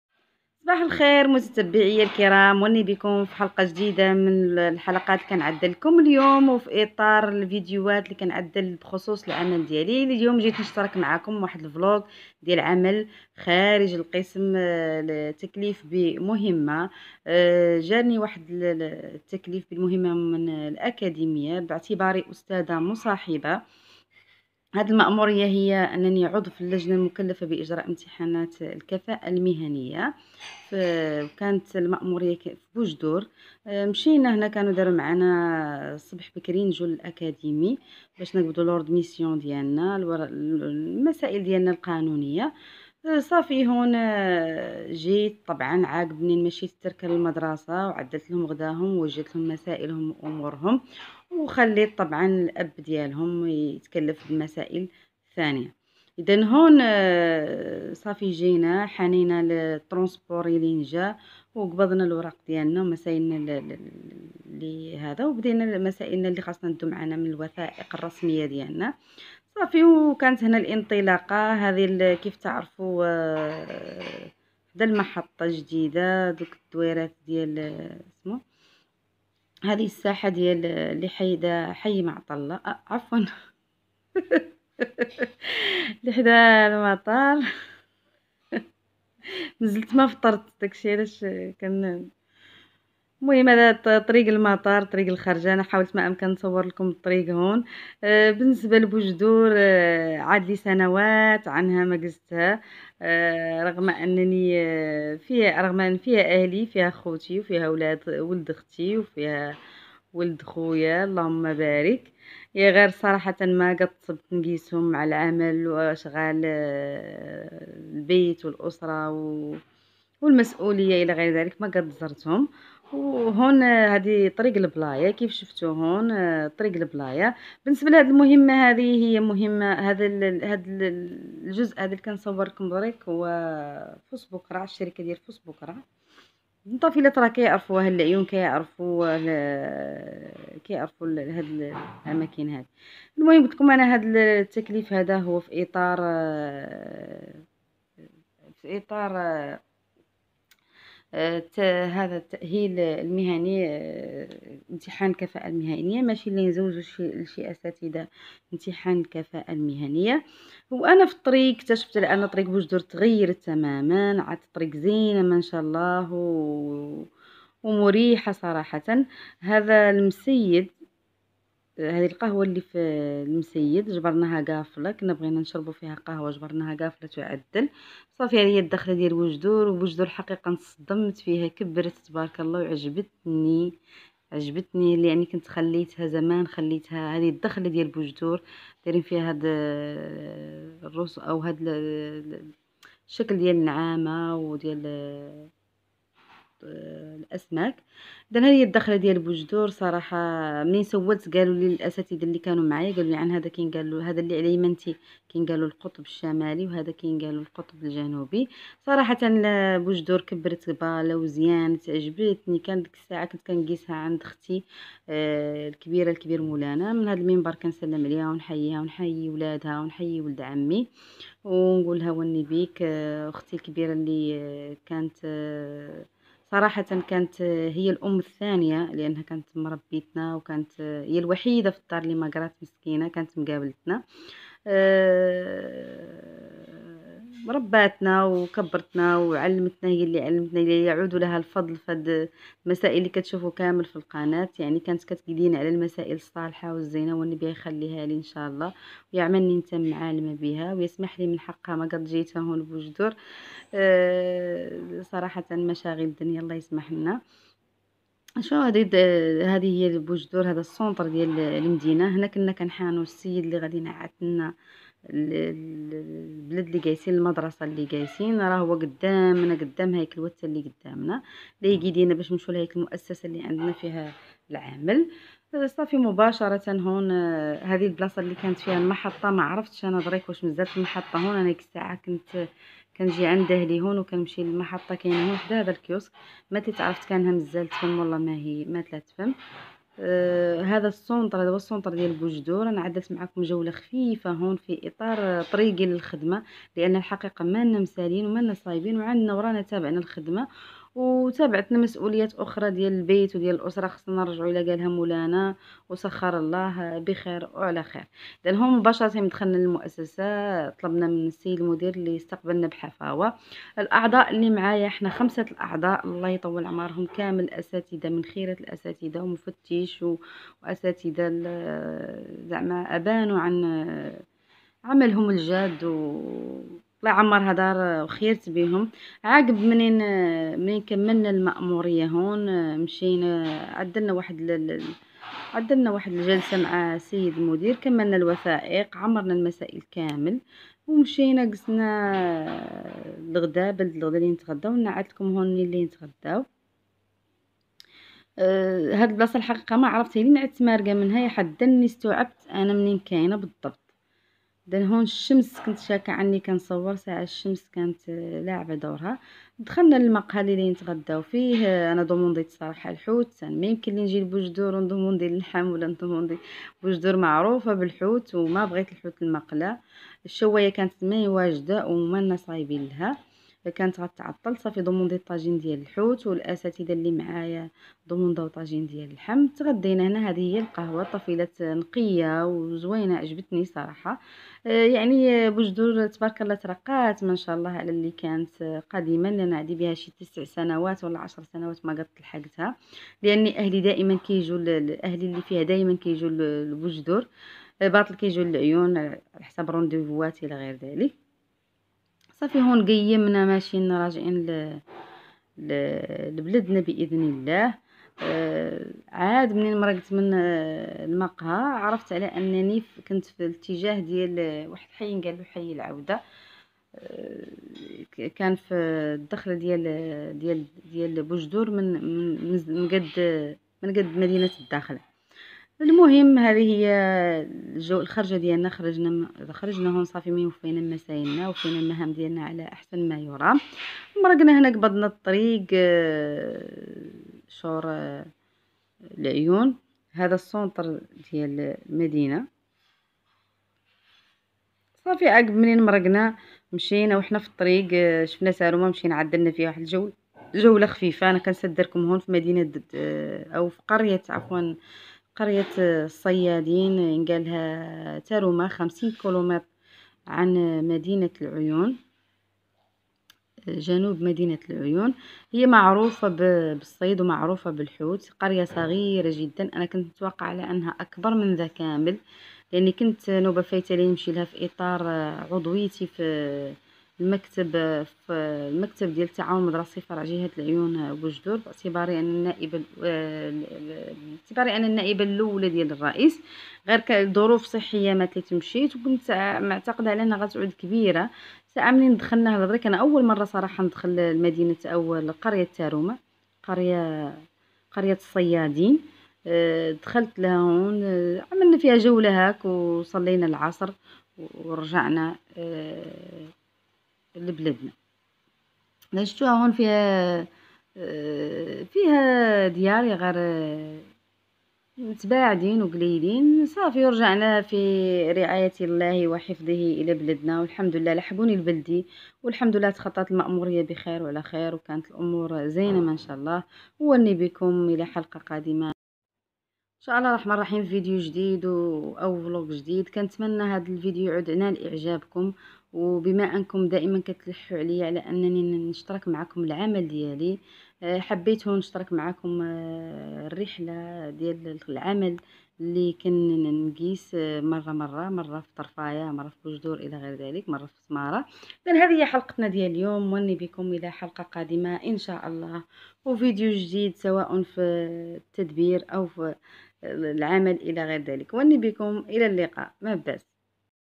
فلوقvlog:بوجدور تنادي أول مرة على القناة نشترك معكم جانب من عملي خارج الفصل القيام بمأمورية
تنظيم وطبخ وتدبير تفرح أم البنين
فيديو يوضح جانب من المسؤولية في عملي كيف أوفق بين بيتي وعملي خارج مدينة العيون
القيام بمأمورية
المديرية الإقليمية لبوجدور
مدينة بوجدور
اكاديمية العيون بوجدور الساقية الحمراء
المسيد 70كلم شمال بوجدور
تاروما قرية الصيادين 50كلم جنوب مدينة العيون
فوسبوكراع